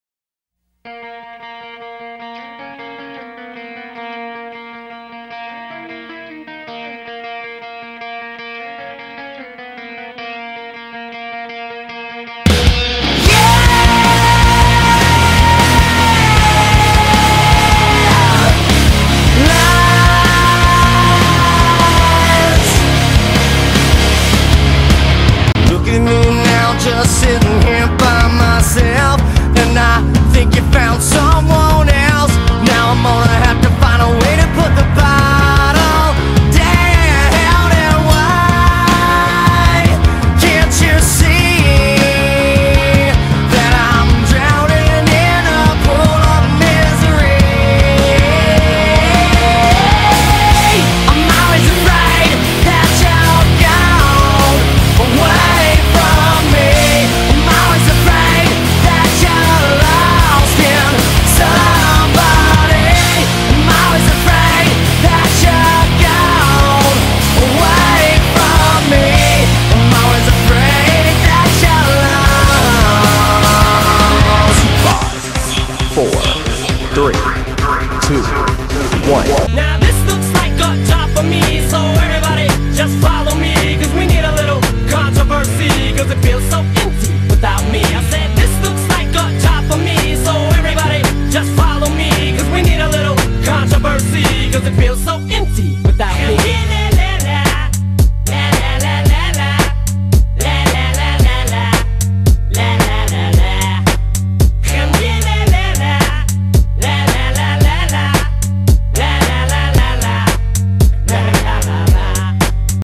away from me (JOshley )
I no ive been going alot of ramys lately but i love them so heres a joshley for youu

plot Joe think ashley is cheating and she assures him that shes nott